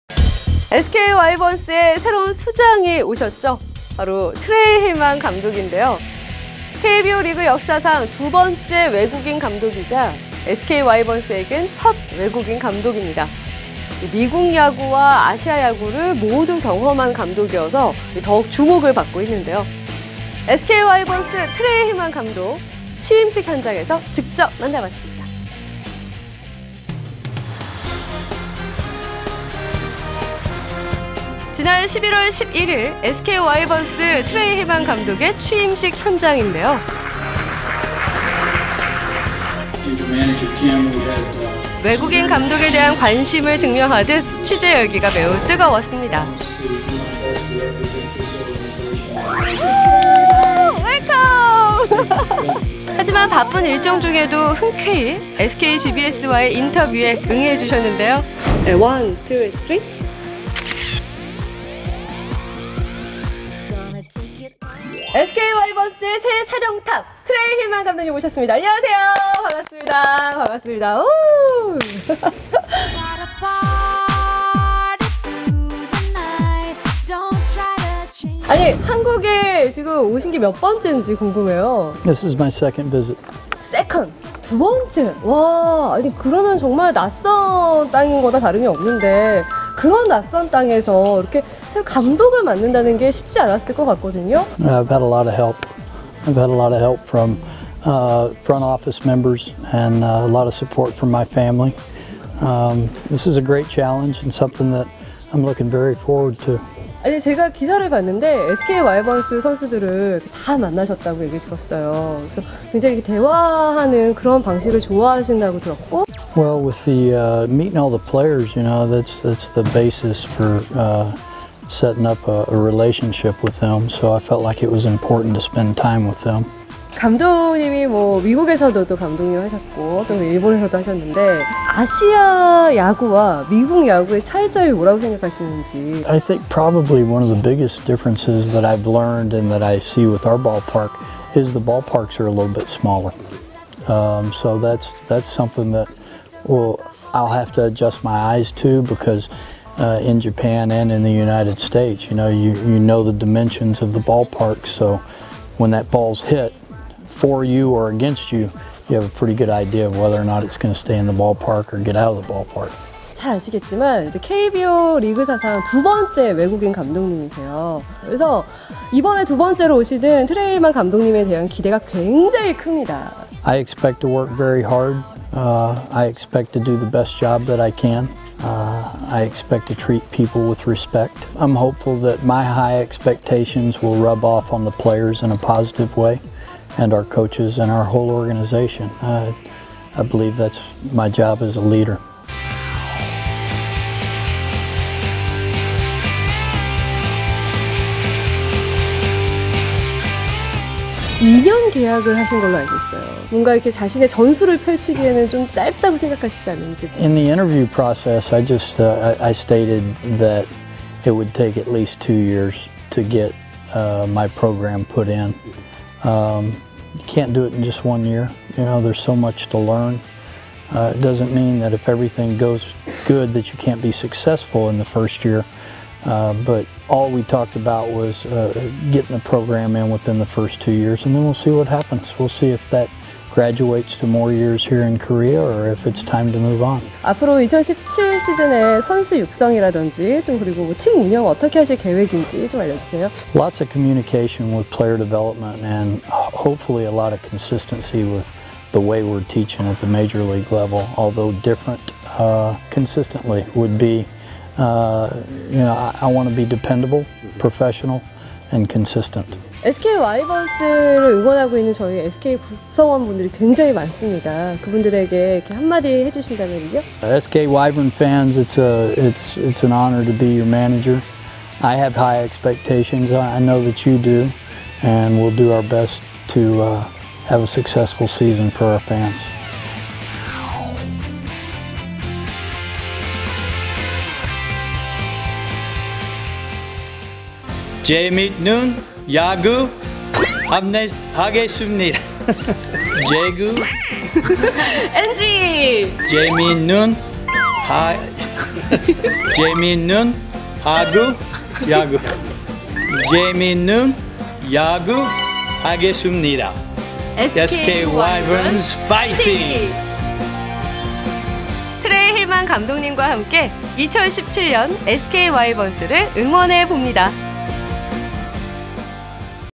SK와이번스 트레이 힐만 감독 취임 인터뷰 | SK gbs
SK그룹방송에서도 트레이 힐만 감독의 취임식 현장을 방문했습니다.
진중함과 유쾌함을 넘나드는 트레이 힐만 감독의 인터뷰 영상입니다.^^